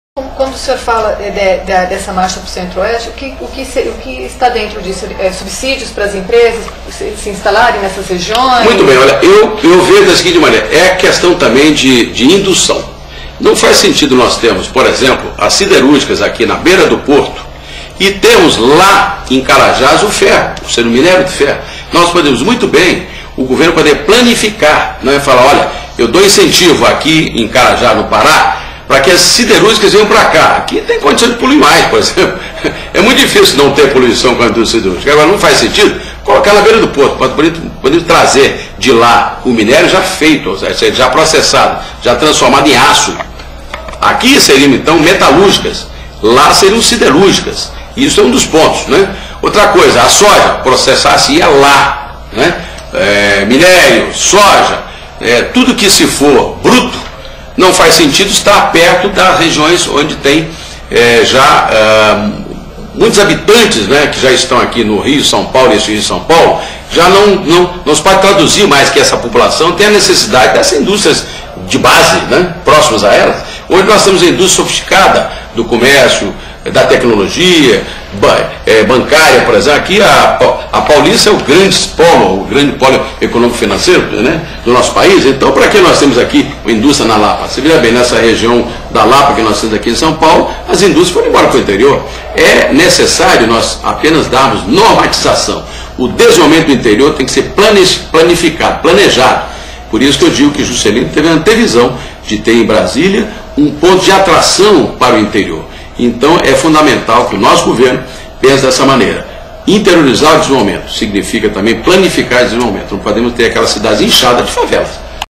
Levy Fidelix tem projetos inovadores para o centro-oeste do país
O candidato a Presidência da República Levy Fidelix propõe para o povo propostas inovadoras.

ou procure por Levy Fidelix junto ao Youtube.

1 - Mudança do Modelo de Desenvolvimento Nacional - Reforma Urgente e Já.
2 - Propositura de mini reforma Constituicional.
3 - Transformação do Bolsa-Família e outros programas sociais em Salário Família.
4 - Eliminação de Impostos sobre dez alimentos da cesta básica, essenciais à vida.
5 - Título Cidadania, Brasil 21.
6 - Criação do Banco de Poupança, Emprego e Desenvolvimento Econômico da Juventude.
7 - Marcha para o Centro-Oeste.
8- Saúde, Educação, Habitação e Segurança.
9 - Revolução da Infraestrutura Nacional.
10 - Brasil Potência do Século XXI.